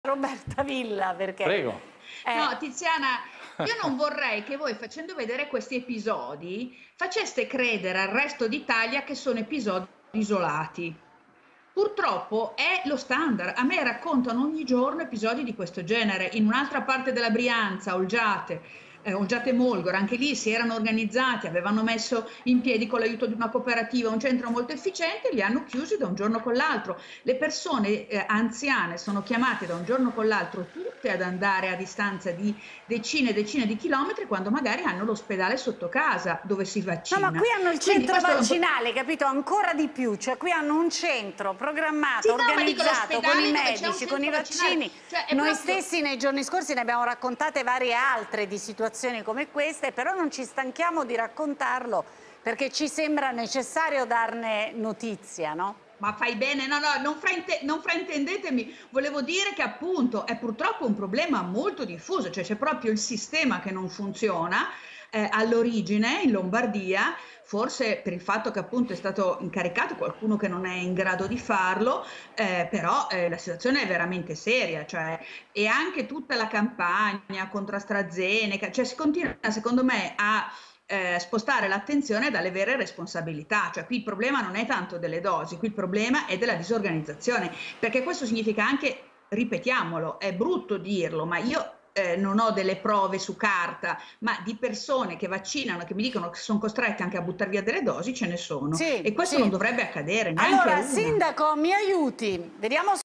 Vaccini in Lombardia, Villa: "Il caso di Verano Brianza non è isolato, anzi è lo standard"
"Gli esempi sono tanti, è il sistema che non funziona" spiega la giornalista scientifica